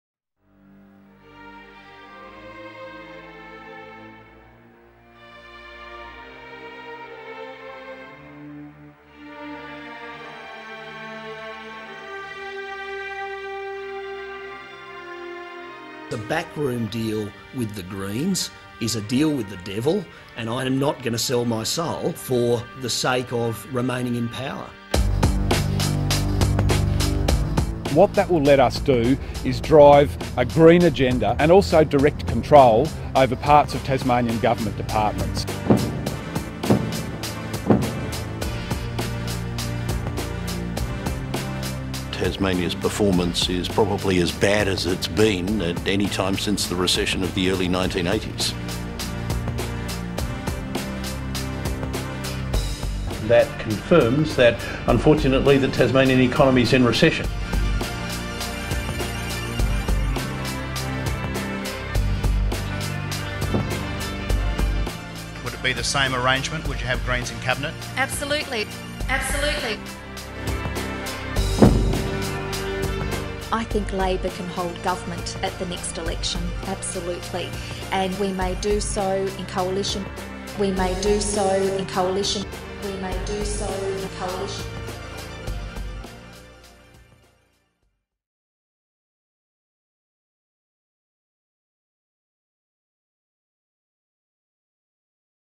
Absolutely
Lara Giddings said today that she wants Tasmanians to "forget" about the last four years. Tasmanians should not forget.